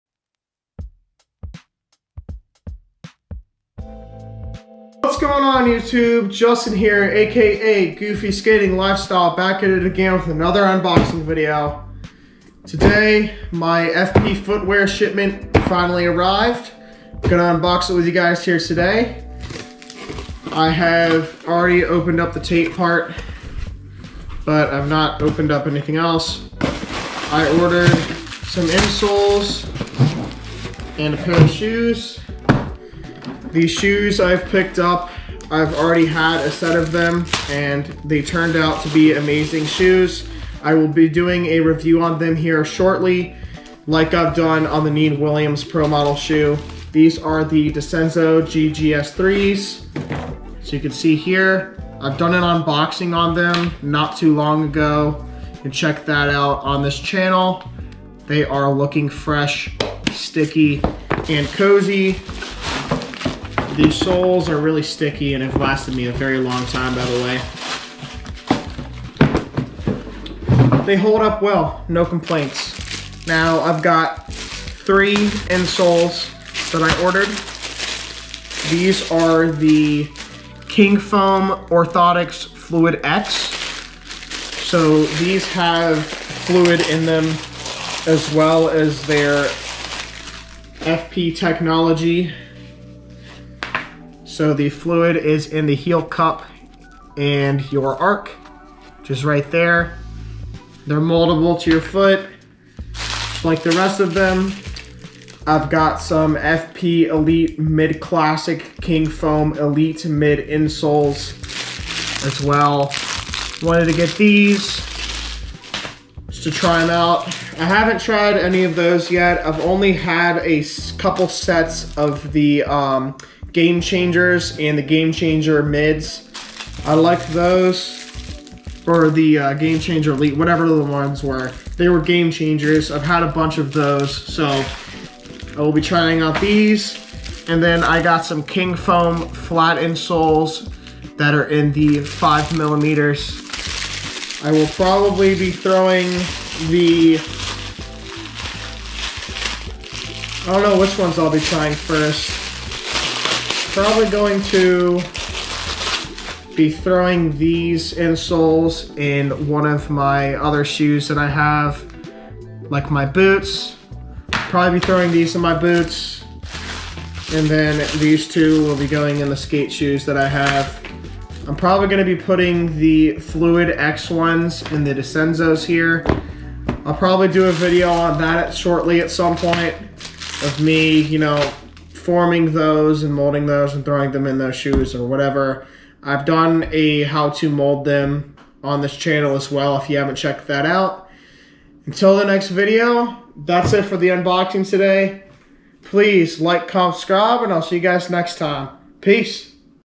Fp footwear unboxing
These are my favorite insoles, the ones I have here today you do not need to mold, the gamechangers you mold forgot to specify that, check my gamechangers how to mold if you have the gamchangers, or on their website, and i recommend them to anyone.